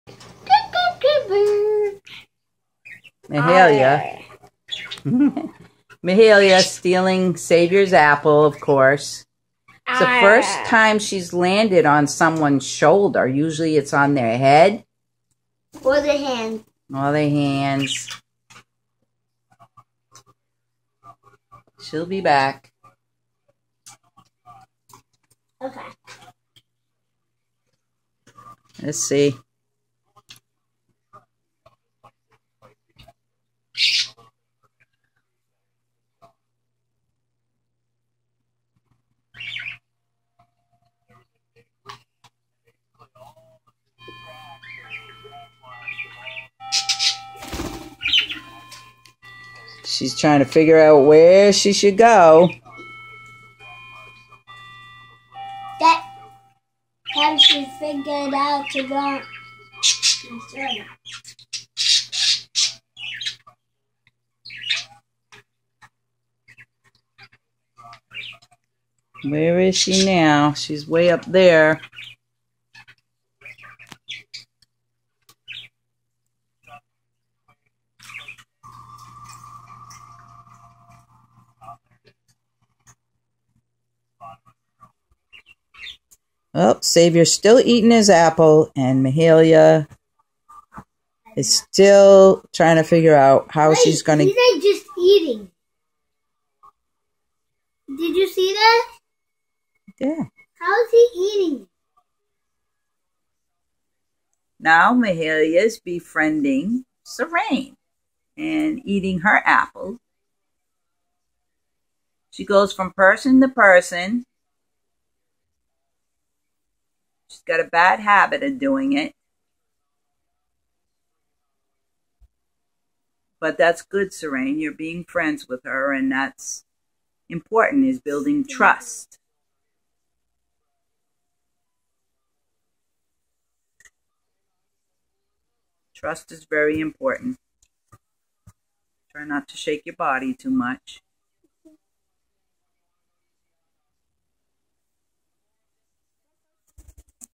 KidsFun! Sav & Mahaileyah-Nov2014
SaviorBruno flocks together with the Pesty Mahaileyah the Parrot, and gives into sharing with his feathered friend.

Produced by: A7P/Kids Crew Productions
November 2014